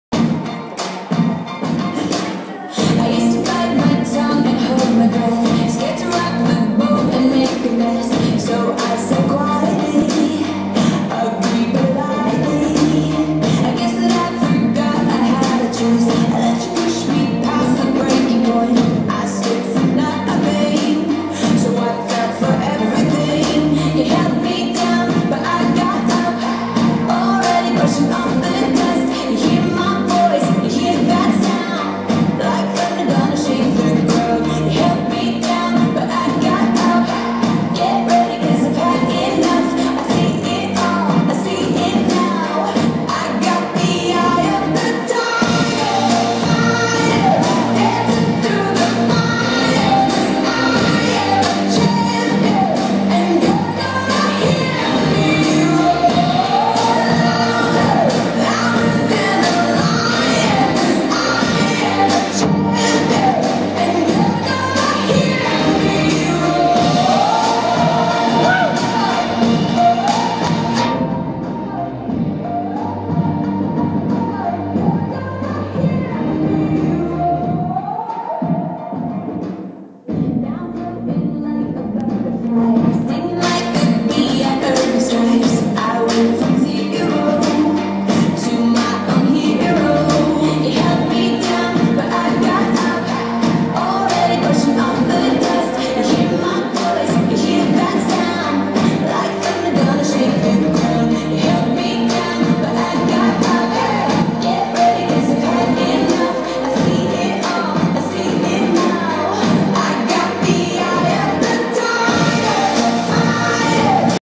Toni K as Katy Perry
Hollywood casino Celebrity impersonator dealer auditions December 2013